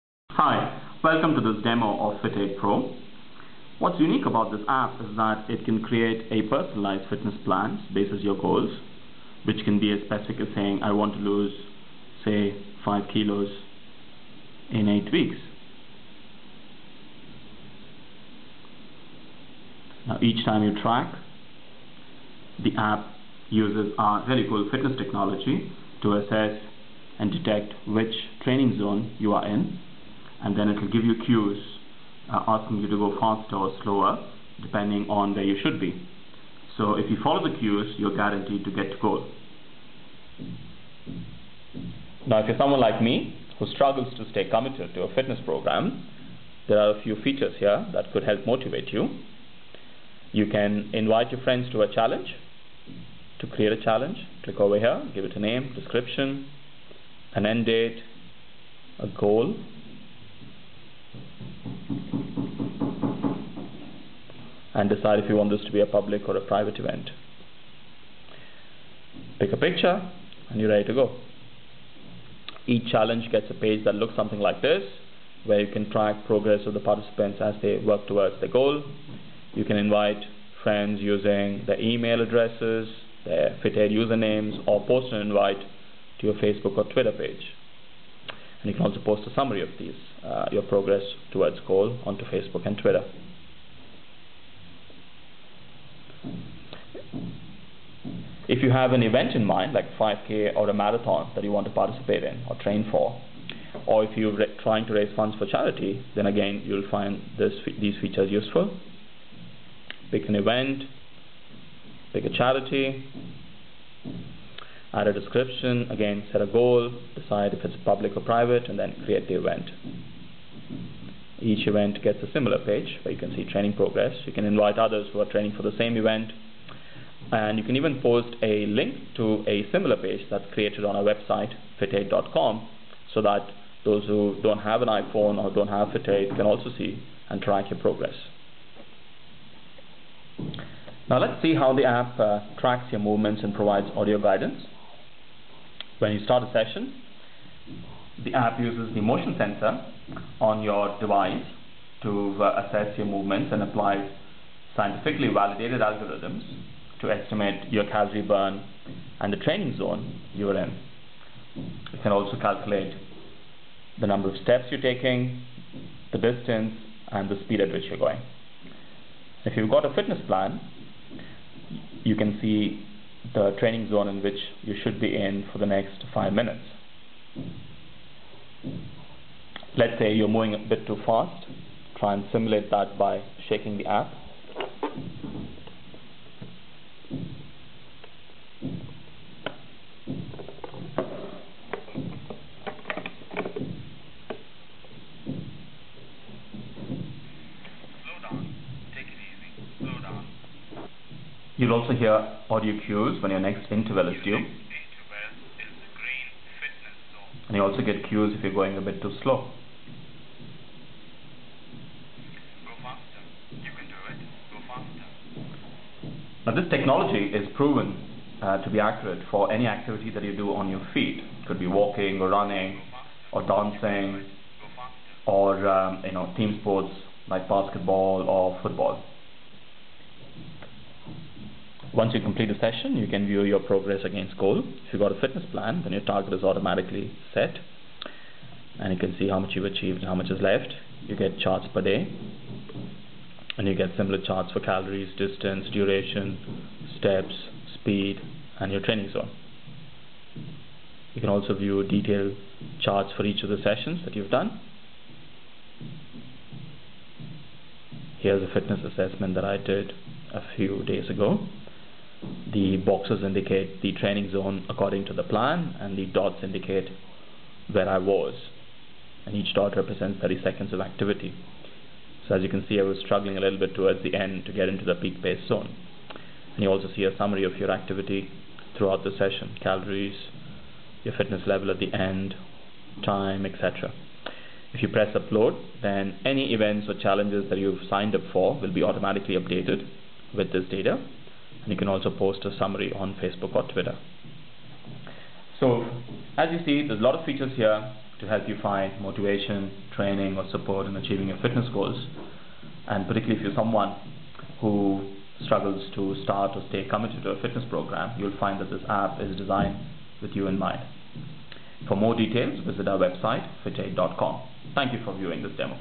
Fitaide Pro iPhone App Demonstration Video
Fitaide uses cutting-edge technology to track your activity and help you find motivation, training and support in achieving your fitness goals. It works for users at any level of fitness and is particularly suited for those who find it difficult to start or stick to a fitness program.